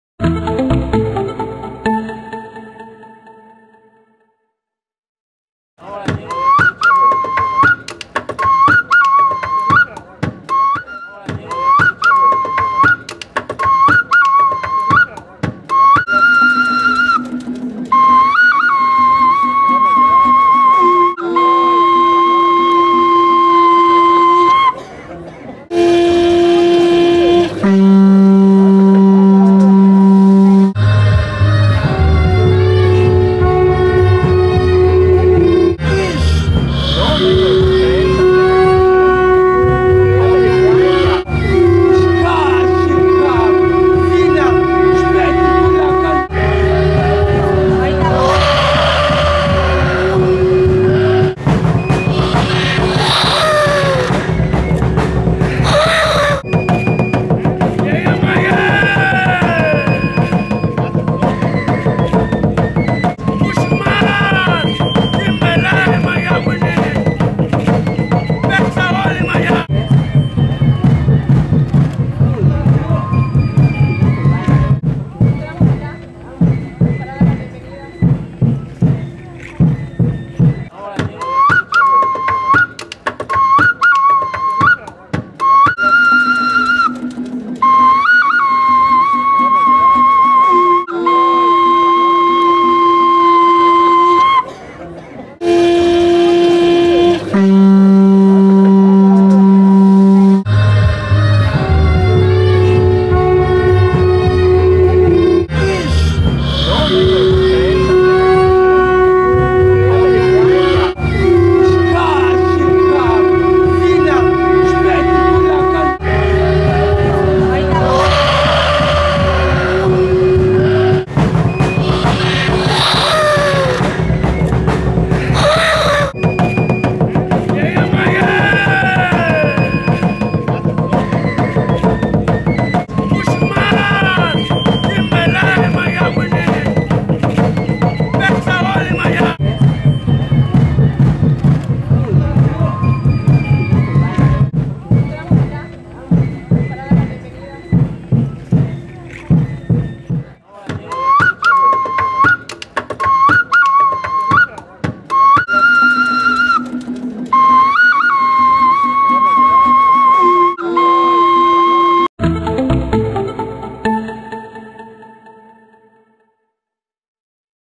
Presentación de la FicMaya 2015
Aspectos de la presentación de la FicMaya 2015 en Uxmal